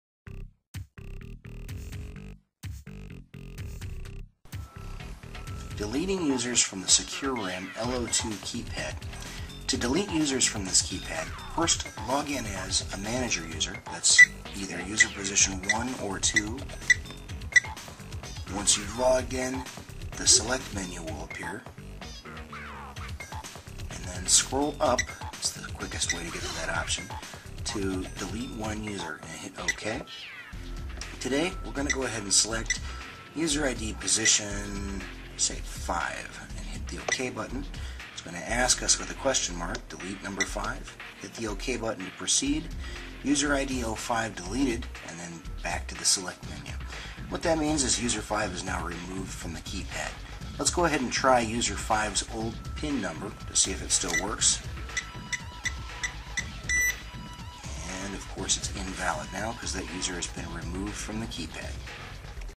SecuRam ECSL-0601A-L02 Keypad (SR2) Deleting Users
Learn how to delete users on the SecuRam ECSL-0601A-L02 Keypad.